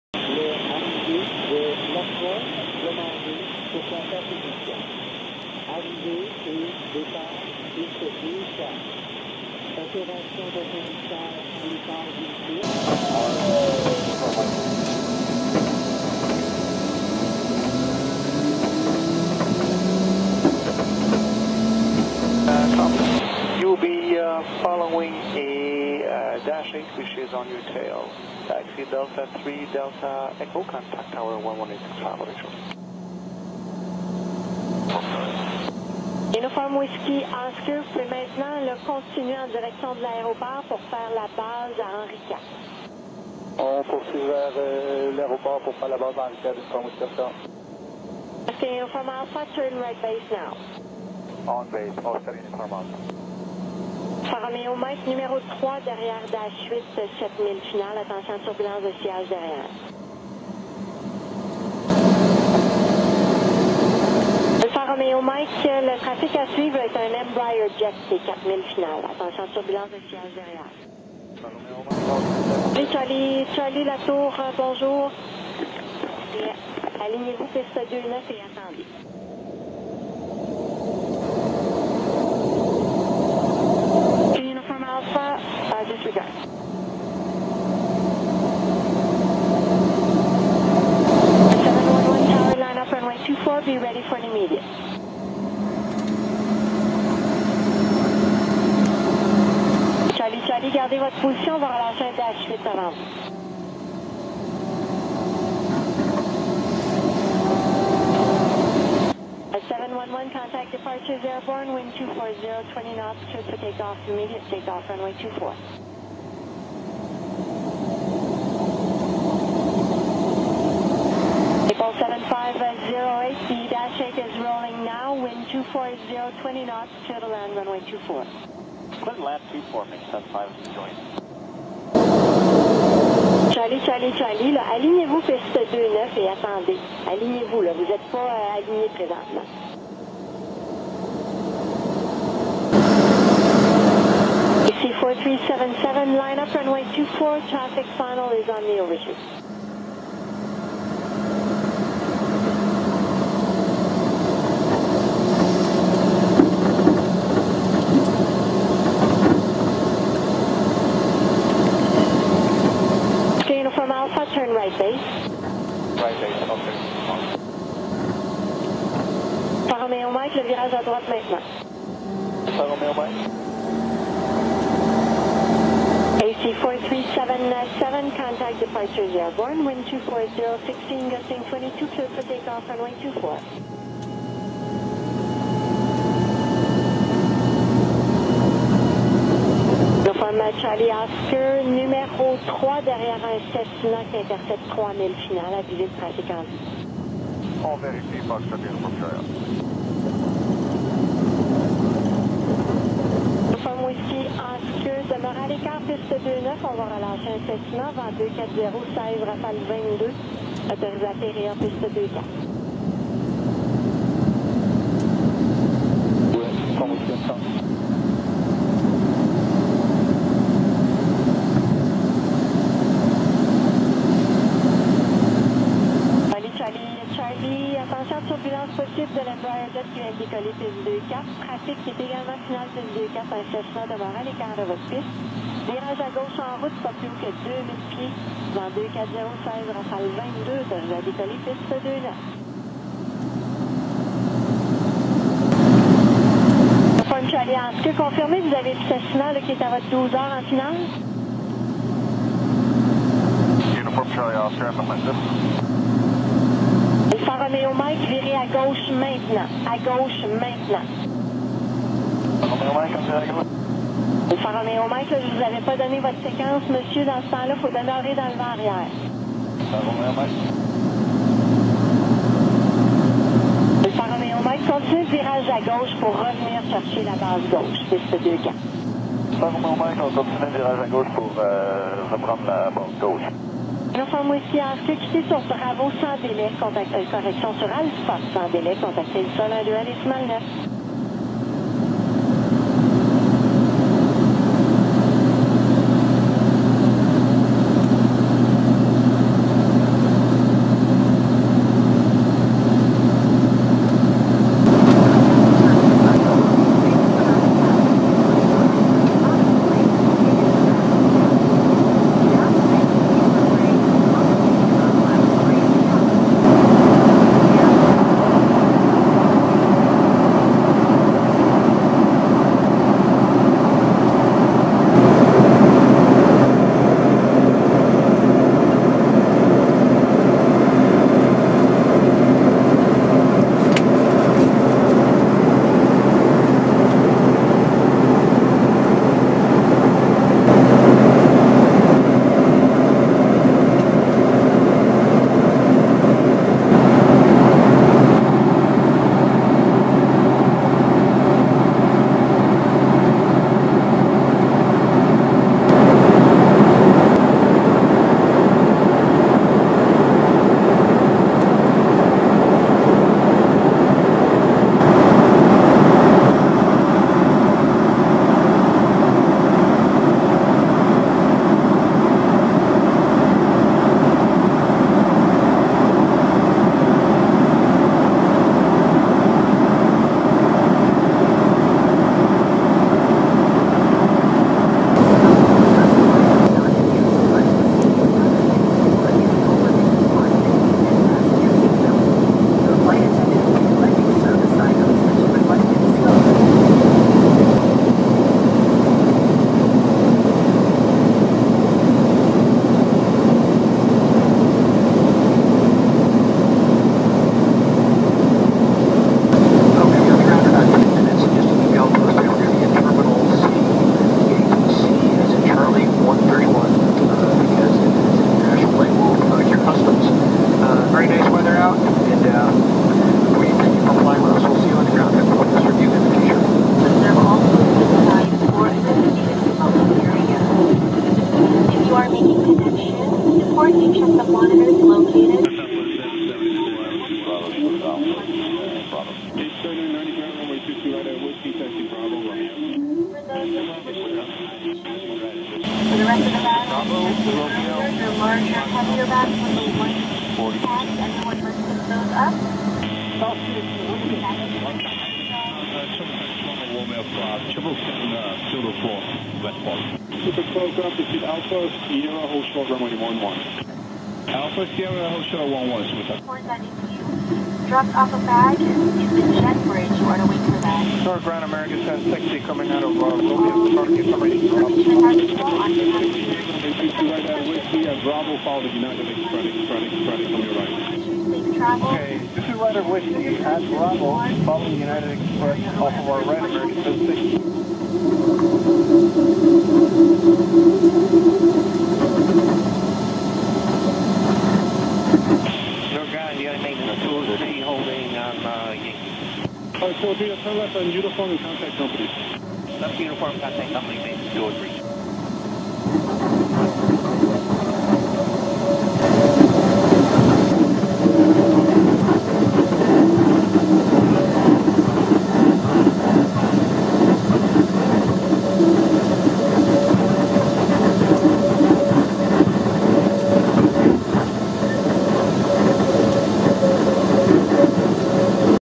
Trip Report - Quebec to Newark with French ATC Audio, United ERJ 145
We follow UA 4377, operated by ExpressJet (call sign "Acey 4377"), from Jean Lesage International Airport (YQB) to Newark Liberty International Airport (EWR).  Includes French language ATIS and tower audio at YQB.